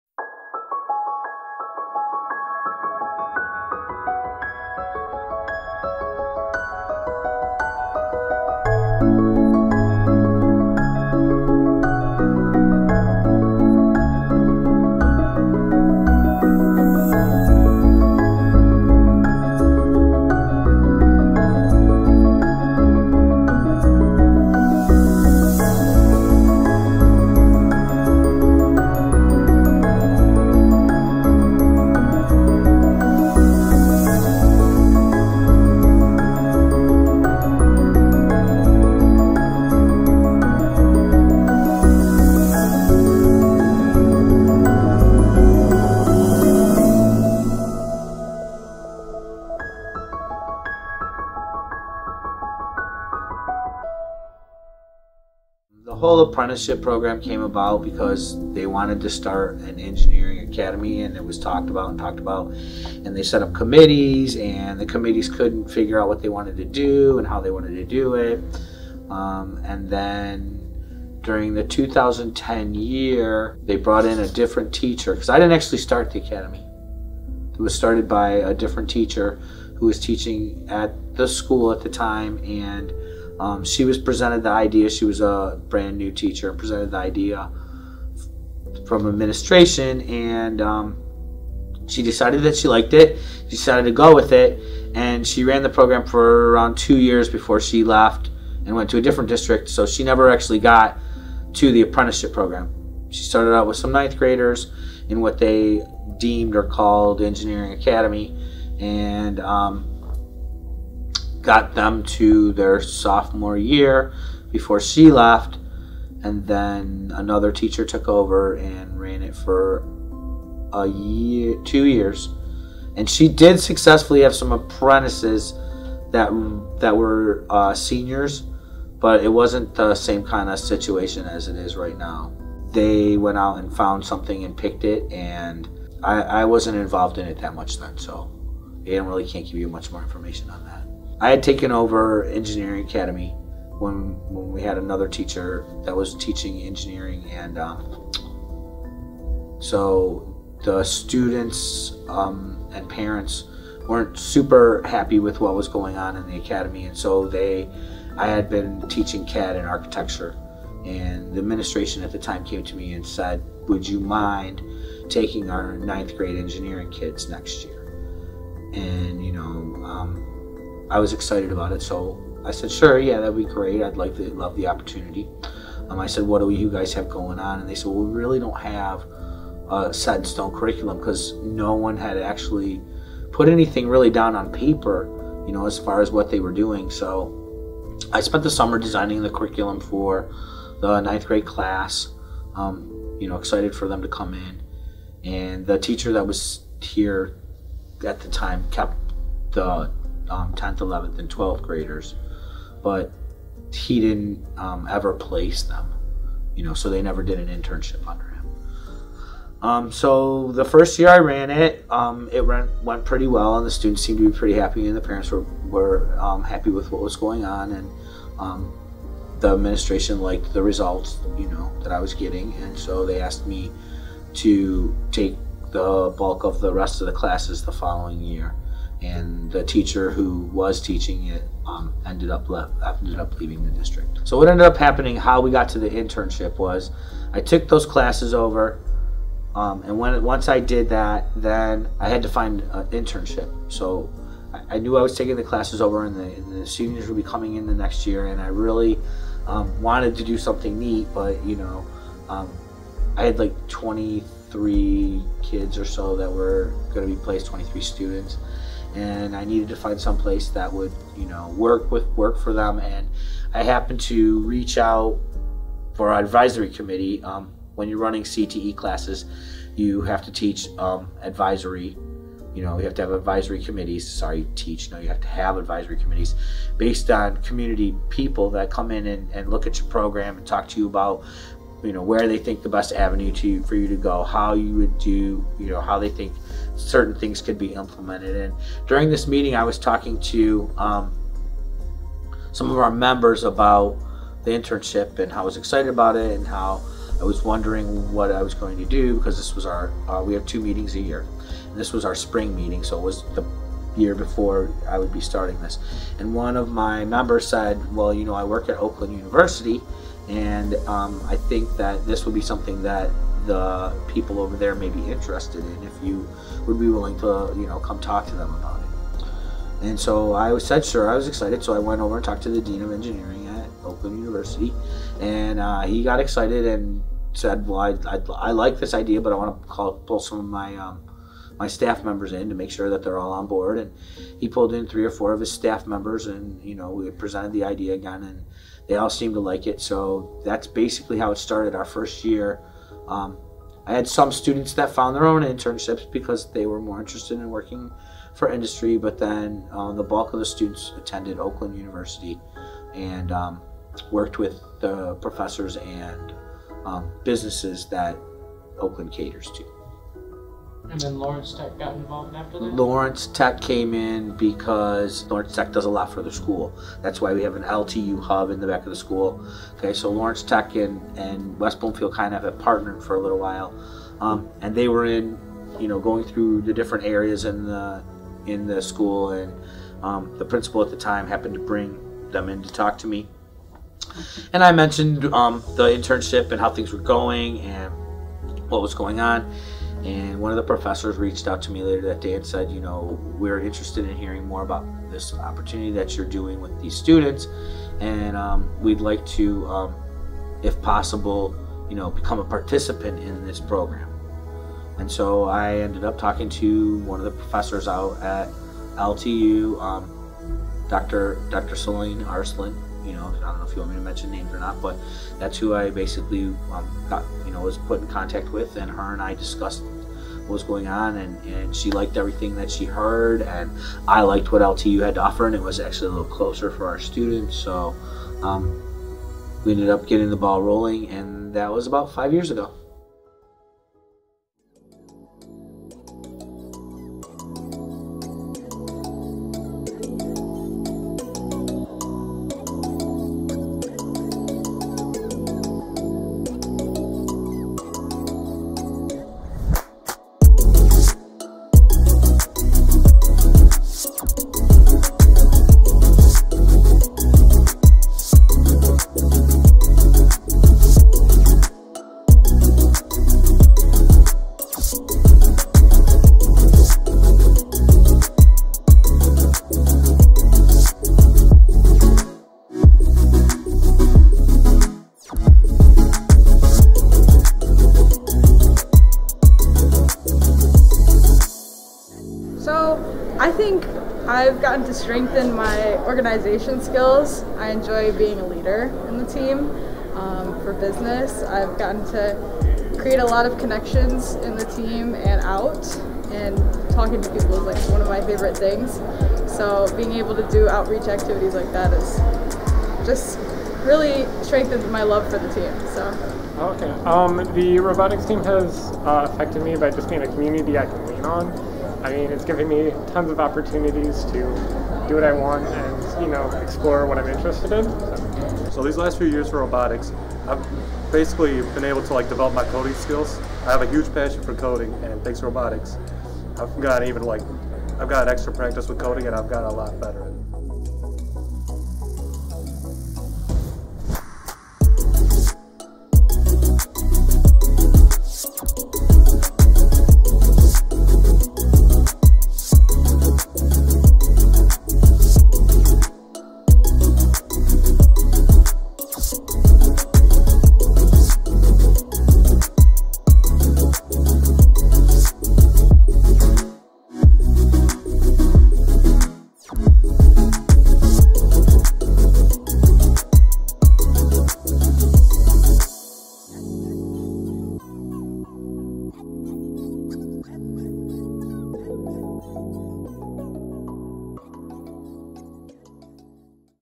Low Budget Short Documentary [ENGINEERING EXPERIENCES]
This project was an open ended project. The teacher of my film class let us go out and do whatever we wanted to do in terms of content (school appropriate, obviously). This really let me get in touch with old friends and get to experience some of my new (at the time) equipment. This was more of a "Run and gun" style shoot, considering I had minimal time on each location to set up and shoot different angles. I had a maximum of two hours on site at the LTU Mechanical Lab and I had five hours total at the Atomigators' Robotics Competition. Overall, spending the time to set up bulky equipment would have kept me from getting the most out of each event.